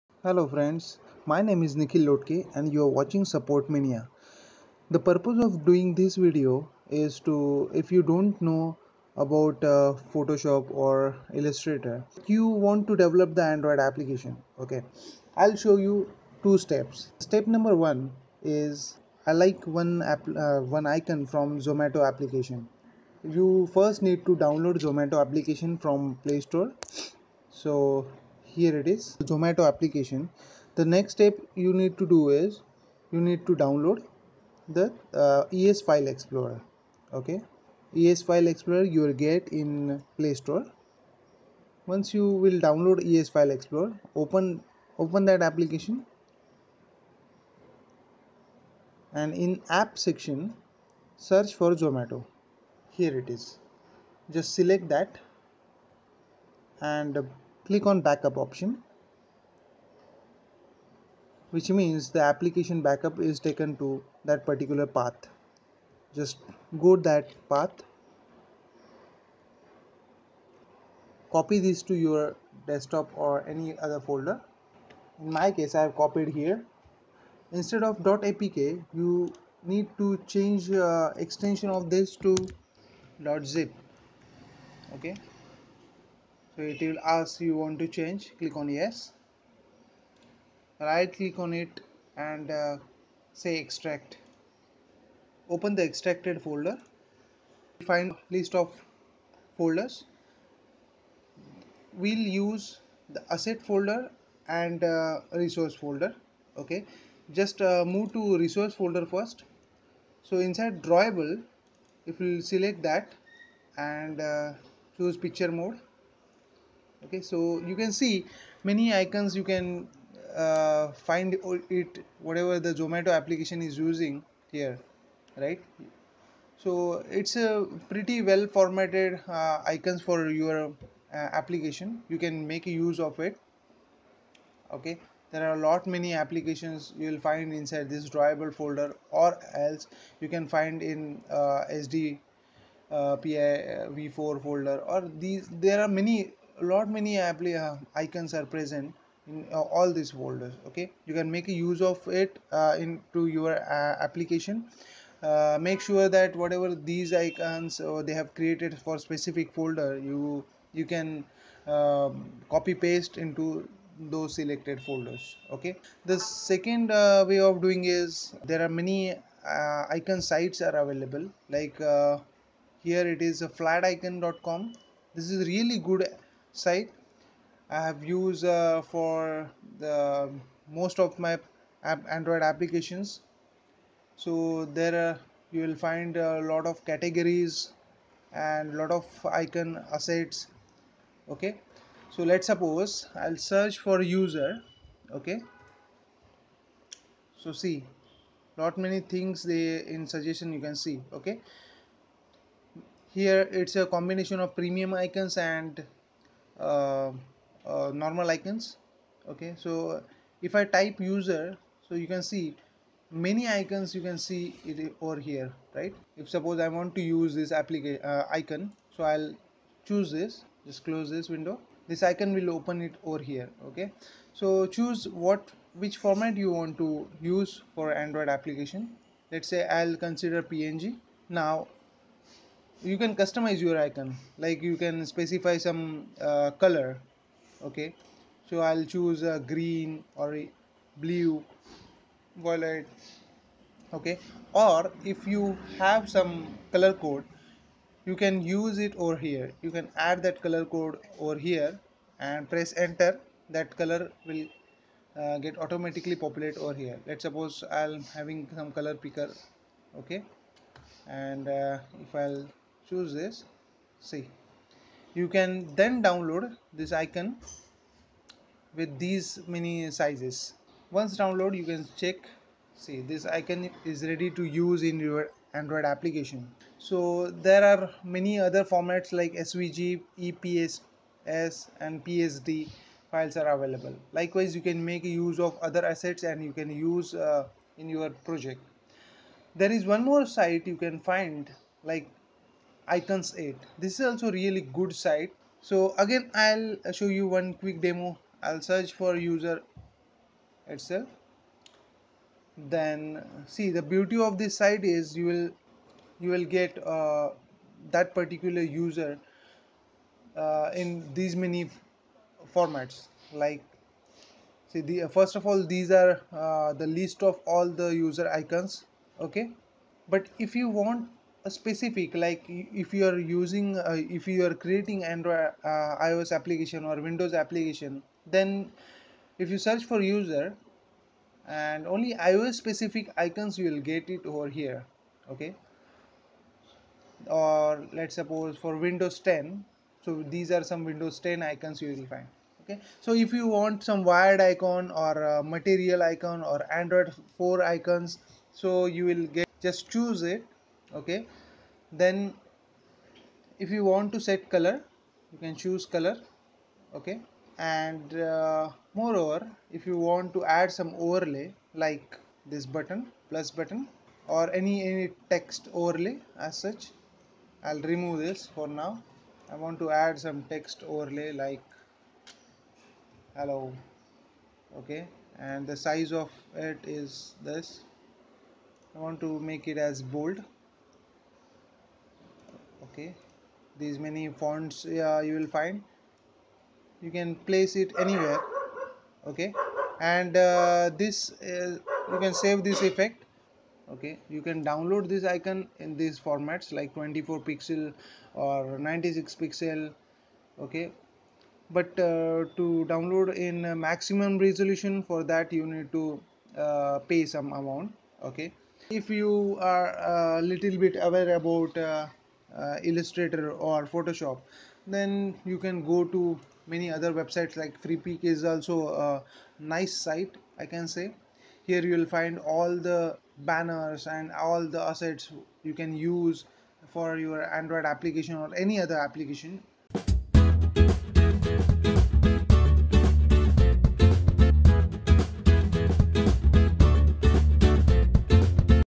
Create Awsome Android app UI without Photoshop
Hello friends, tried my best to explain you without Photoshop or illustrator knowledge still you can able to create awesome android app user interface.
Watch till end so that you will also feel that you don't need any designer so as to polish your android app.